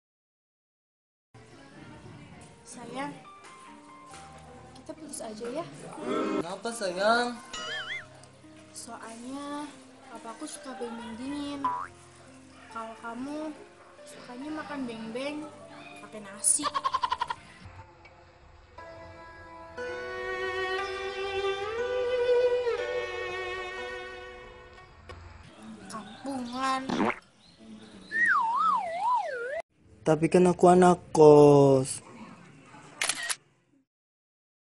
Laki - laki ini nekad melakakukan hal ini
Makan nasi sama beng beng gaes bayangin aja, 😅 mantoelll dahhh